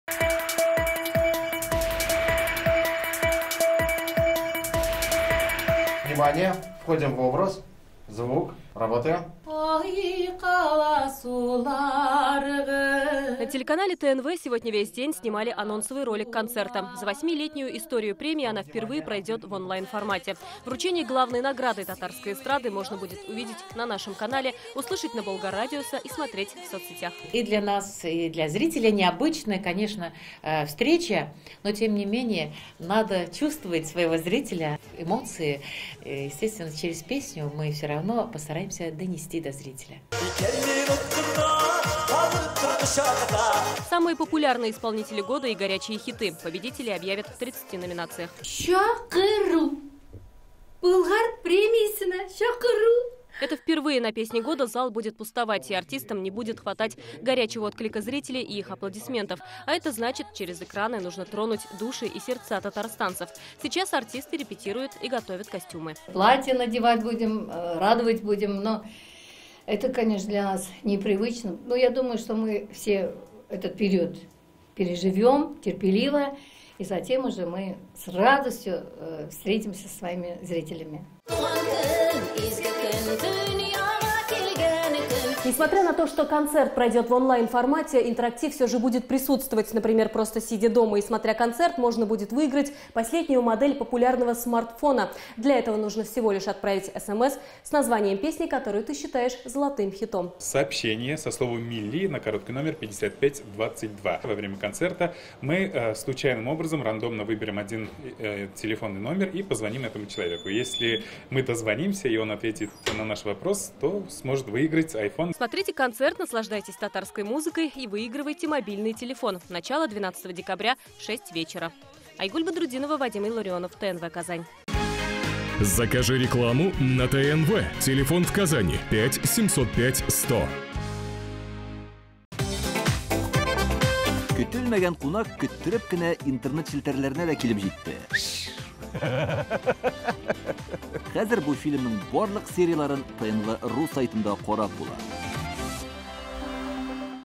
12-го декабря в Казани пройдет ежегодная церемония вручения музыкальной премии "Болгар радиосы"
Ссылка на видео:

12-го декабря в Казани пройдет ежегодная церемония вручения музыкальной премии "Болгар радиосы". Несмотря на то, что долгожданный концерт будет в онлайн формате, организаторы обещают интерактив и возможность выиграть смартфон. Где и как смотреть концерт и участвовать в конкурсе? Как артисты готовятся к главной музыкальной премии года - расскажет Айгуль Бадрутдинова.

Координаты:
☎ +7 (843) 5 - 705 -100